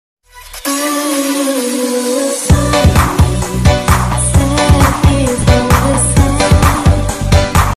Lets see if you know me! (LOOK IN DECRIPTION) (inspired)
Hai
Its Jay!!
Lets see if you know me well!
And maybe you can make your own quiz!